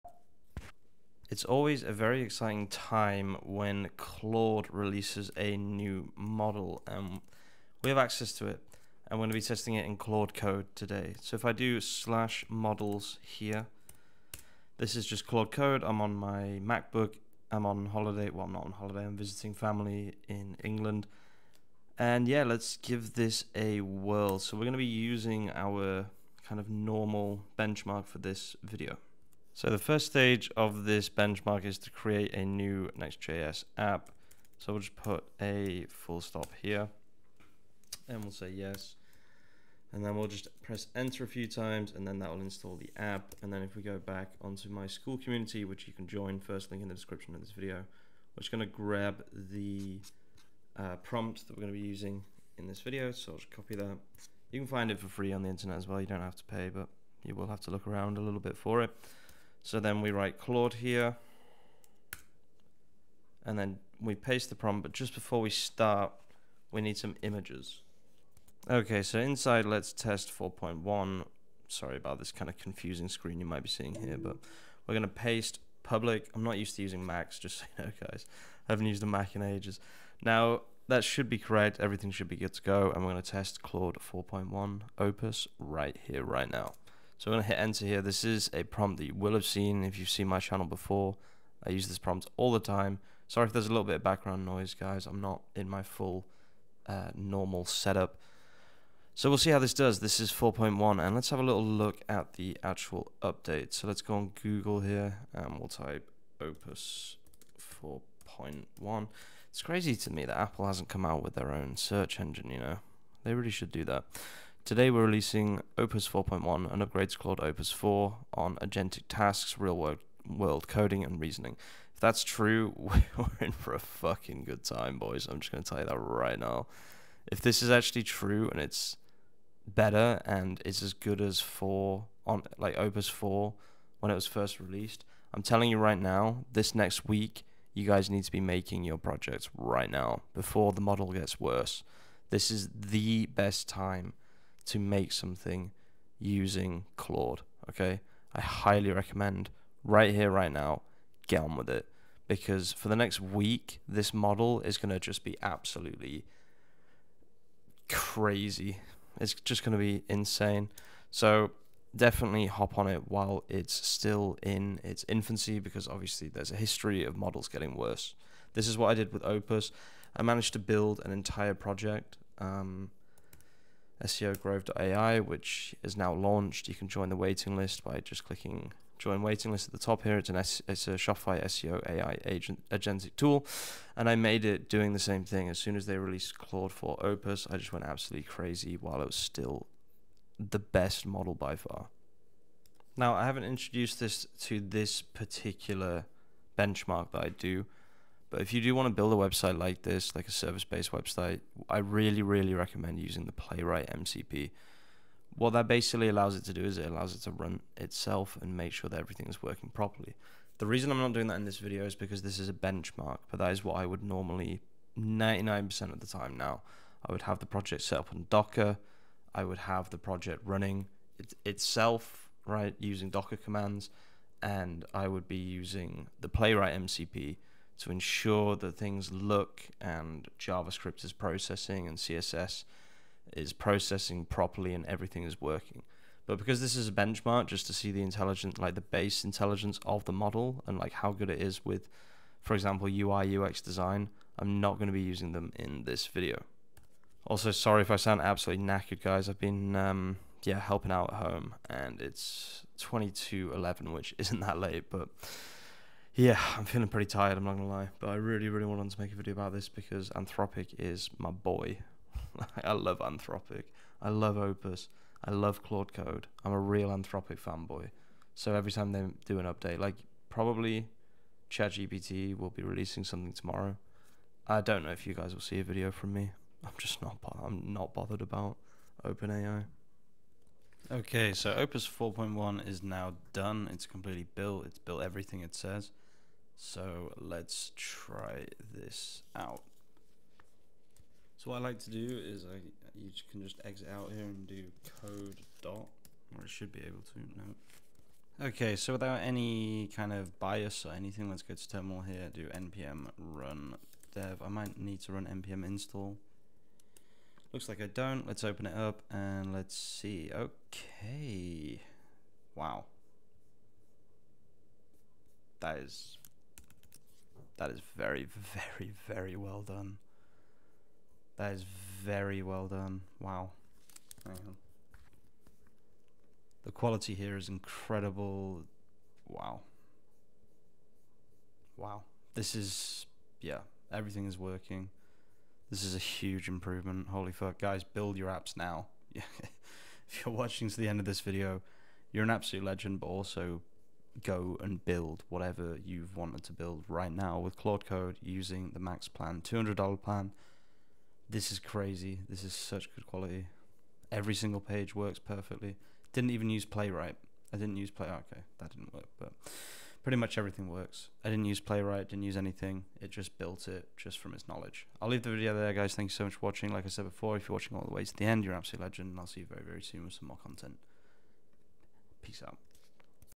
This Opus 4.1 Update Just Made Claude Code INSANE
Let's test the latest Anthropic model - Opus 4.1, which is the new BEST MODEL IN THE WORLD - Here's my test with the benchmark I always do on all new models, without MCPs, testing their ability to use AI to software develop and create entire websites, apps, and softwares purely from their own knowledge

This is starting to feel like AGI - Claude Code with opus 4.1 is completely nuts